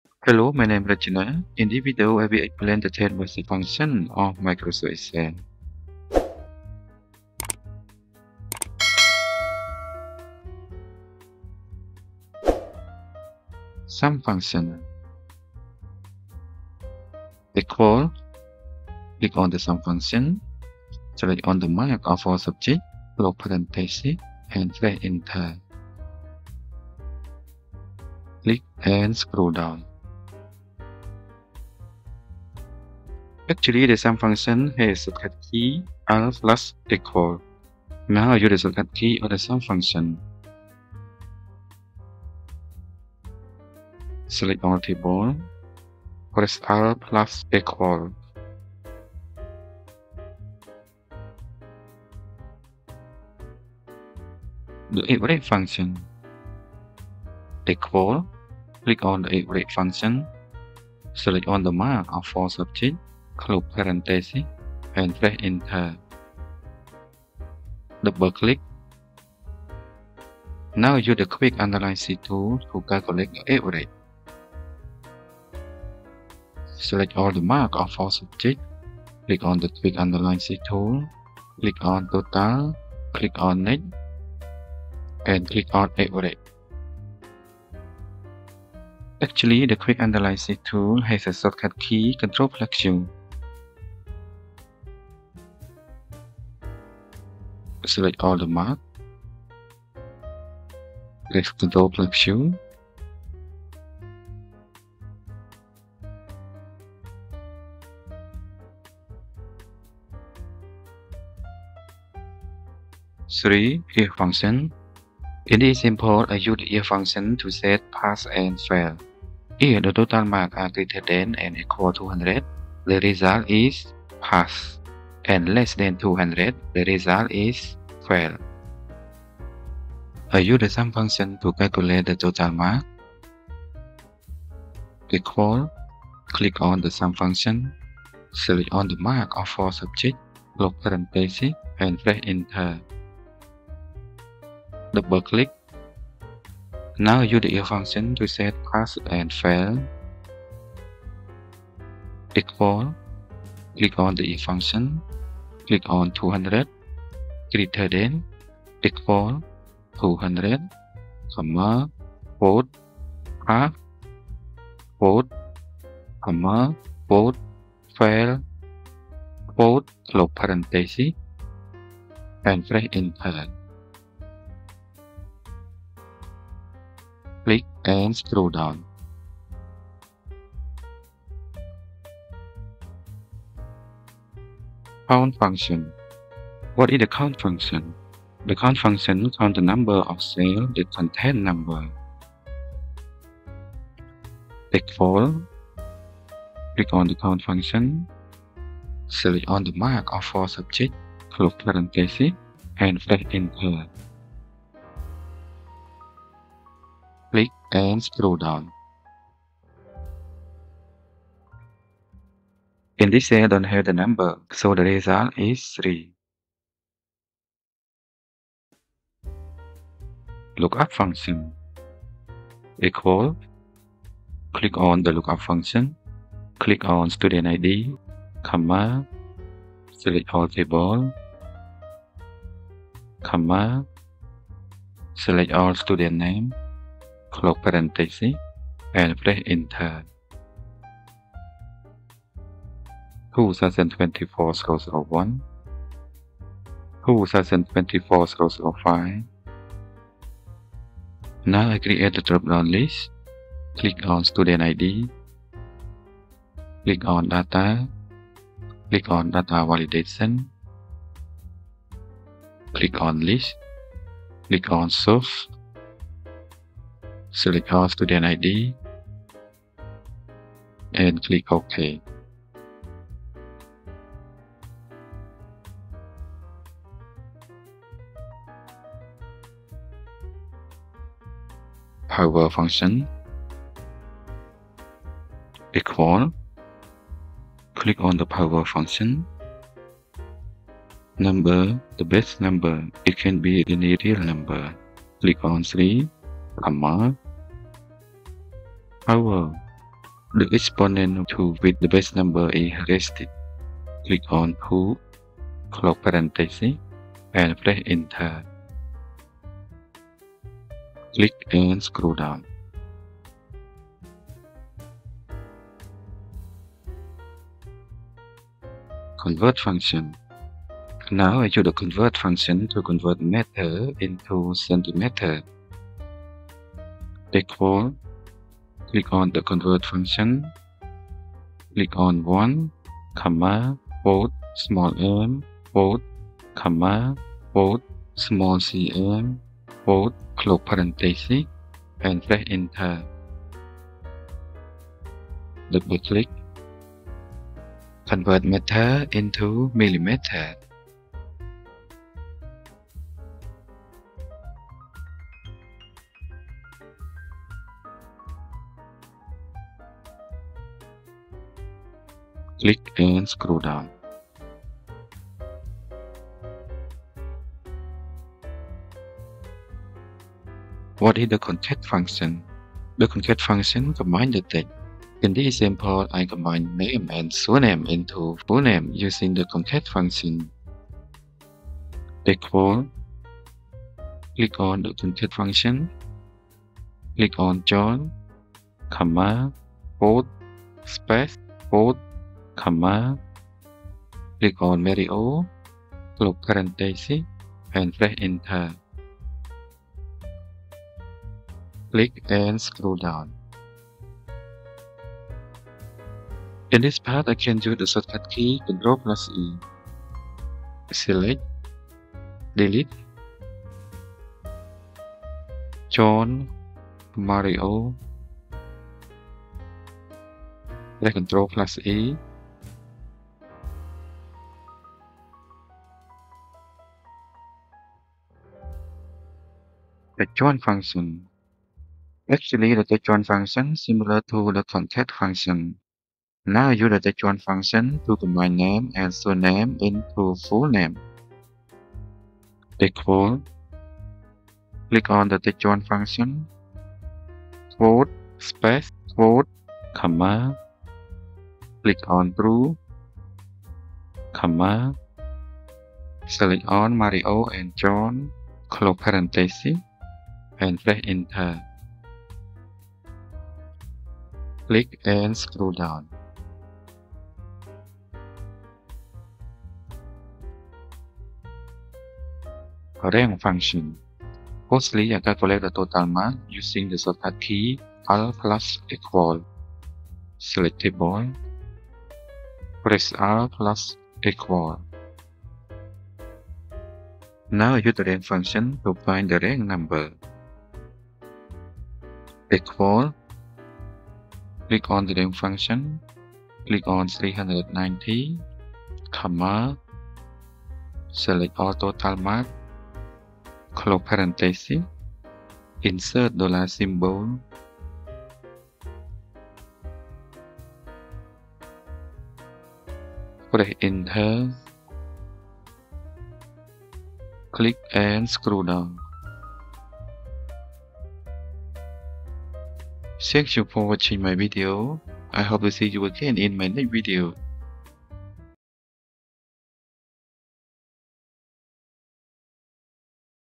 10 Useful Functions for Beginners in Excel – Learn the Most Used Functions First!
10 Useful Functions for Beginners in Microsoft Excel and Make You Expert in Microsoft Excel.

In this video, I explain the 10 Useful functions for beginners in Excel such as SUM, AVERAGE, IF, COUNT, LOOKUP, POWER, CONVERT, CONCAT, TEXTJOIN, and RANK function.

What is the SUM function in Microsoft Excel?
The SUM function adds values. You can add individual values, cell references or ranges or a mix of all three.
Formula in this video:
=SUM(B3:E3)

What is the AVERAGE function in Microsoft Excel?
The AVERAGE function measures central tendency, which is the location of the center of a group of numbers in a statistical distribution.
=AVERAGE(B3:E3)

What is the IF function in Microsoft Excel?
The IF function in Excel is a logical function that compares a value to an expected value and returns a result based on whether the comparison is true or false.
=IF(F3 Greater Than =200,"Pass","Fail")

What is the COUNT function in Microsoft Excel?
The COUNT function counts the number of cells that contain numbers, and counts numbers within the list of arguments. Use the COUNT function to get the number of entries in a number field that is in a range or array of numbers.
=COUNT(B3:E3)

What is the LOOKUP function in Microsoft Excel?
The LOOKUP function in Excel is a tool that matches values in a cell to other rows or columns and returns the corresponding results. It's useful for finding specific information across large sets of data in a workbook.
=LOOKUP(E5,A2:C13,B2:B13)

What is the POWER function in Microsoft Excel?
The POWER function in Excel is a mathematical function that calculates the result of a number raised to a given power. It's useful for performing exponential calculations, especially with large data sets.
=POWER(A3,B3)

What is the CONVERT function in Microsoft Excel?
The CONVERT Function in Excel is an Excel Engineering Function. The function is useful for the conversion of units from one measurement system into another measurement system. For example, CONVERT will help us to convert meter into centimeter, meter into millimeter.
=CONVERT(A3,"m","mm")

What is the CONCAT function in Microsoft Excel?
The CONCAT function combines the text from multiple ranges and/or strings, but it doesn't provide delimiter or Ignore Empty arguments. CONCAT replaces the CONCATENATE function. However, the CONCATENATE function will stay available for compatibility with earlier versions of Excel.
=CONCAT(A2," ",B2)

What is the TEXTJOIN function in Microsoft Excel?
The TEXTJOIN function combines the text from multiple ranges and/or strings, and includes a delimiter you specify between each text value that will be combined.
=TEXTJOIN(" ",TRUE,A3:B3)

What is the RANK function in Microsoft Excel?
The RANK function in Microsoft Excel determines the rank of a number in a list of numbers. The rank of a number is its size relative to other values in the list. The RANK function can also sort numbers in ascending or descending order.
=RANK(F3,$F$3:$F$12)

I hope to see you again in my next video.
If you have any questions, please write in the comments below.
Prepared by: Dr. Rachana Chiv